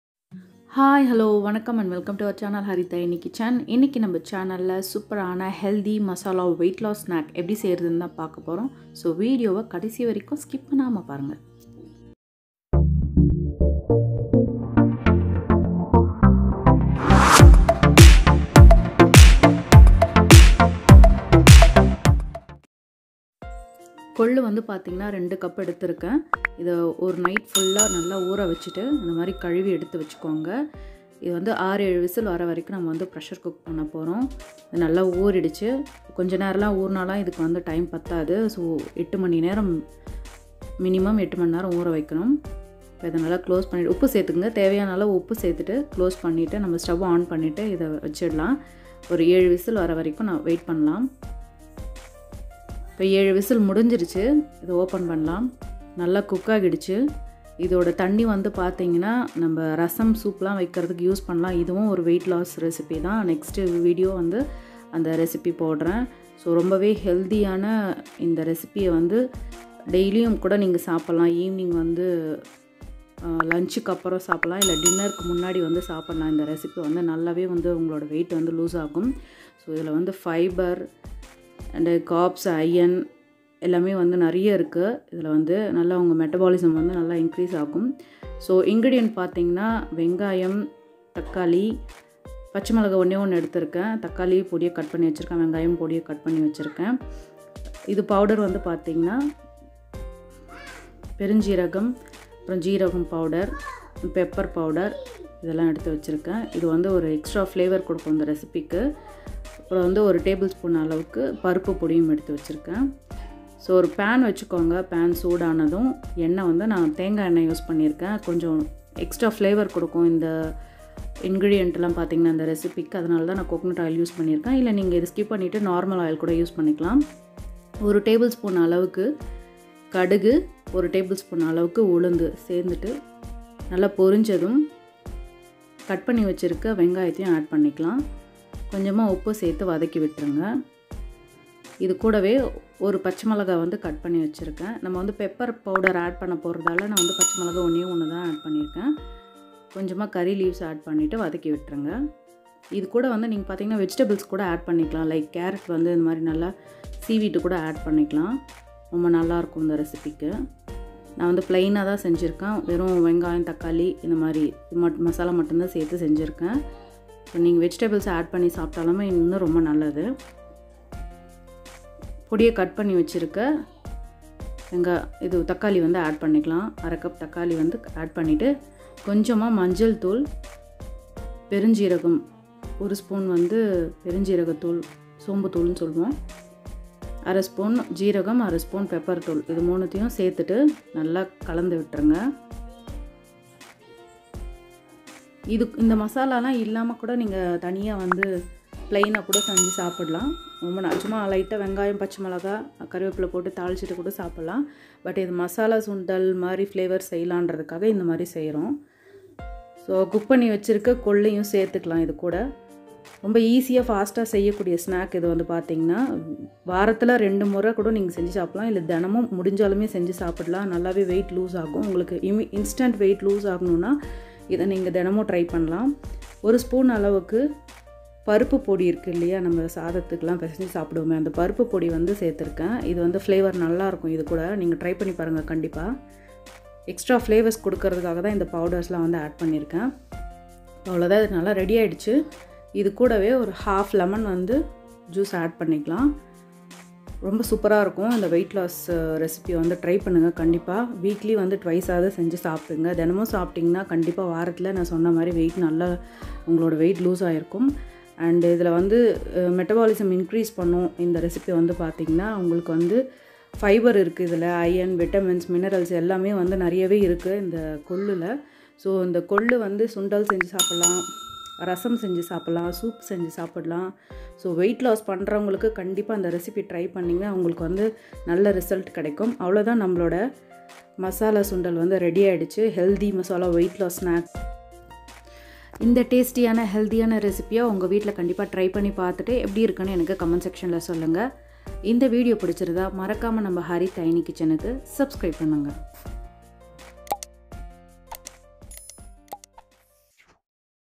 கொள்ளு மசாலா சுண்டல்| Healthy snack recipe for weight loss in tamil|kollu sundal recipe
Best Snack and sundal recipe for weight loss..if u trying to reduce ur weight means pls add this recipe in ur menu...fast weight reducer this horse gram ...this recipe is totally filled with minerals,iron, vitamins and fiber..it helps to increase ur body metabolism. it helps in good digestion. reduce fats and burns ur calories...try this in ur home and leave ur feedbacks at comment section.